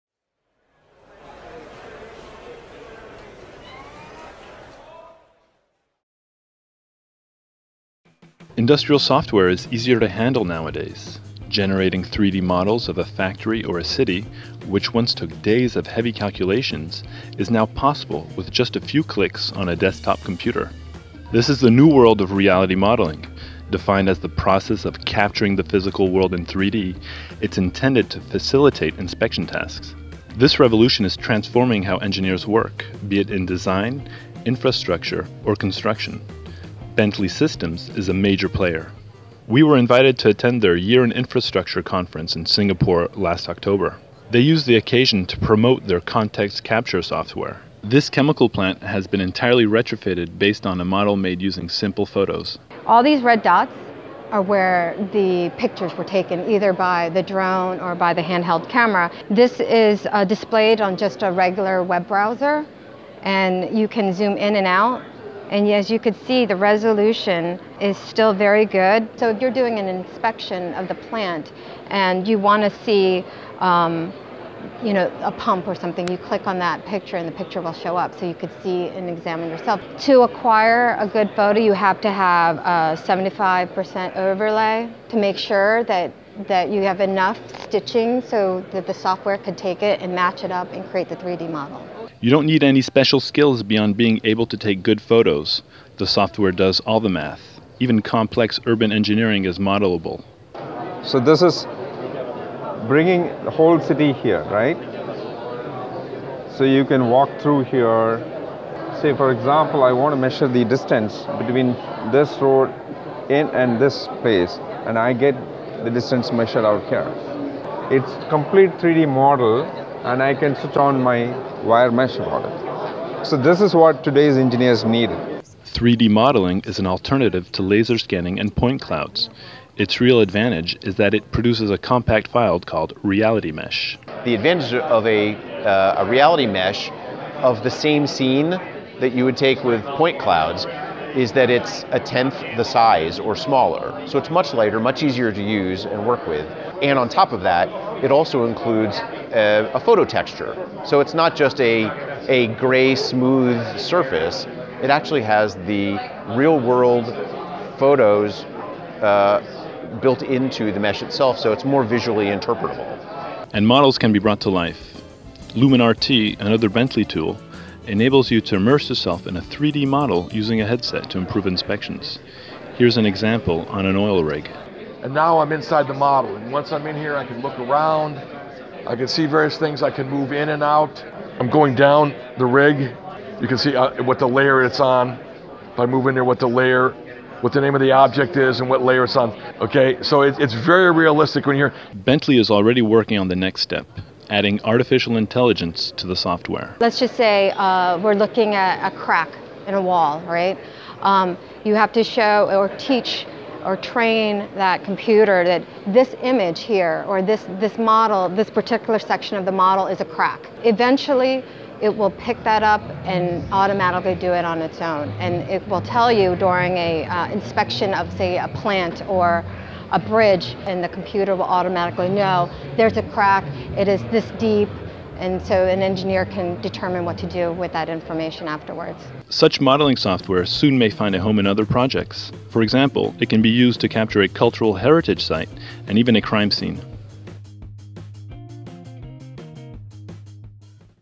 DirectIndustry e-magazine | Reality Modeling is Child’s Play
Subtitles available in French, German, Italian, Spanish, Portuguese, Russian, Chinese and Japanese.

Industrial software is easier to handle nowadays. Generating 3D models of a factory or a city, which once took days of heavy calculations, is now possible with just a few clicks on a desktop computer. This is the new world of reality modeling. Defined as the process of capturing the physical world in 3D, it is intended to facilitate inspection task. This revolution is transforming how engineers work, be it in design, infrastructure or construction. Bentley Systems is a major player.

We were invited to attend their Year in Infrastructure Conference in Singapore last October. They used the occasion to promote their ContextCapture software.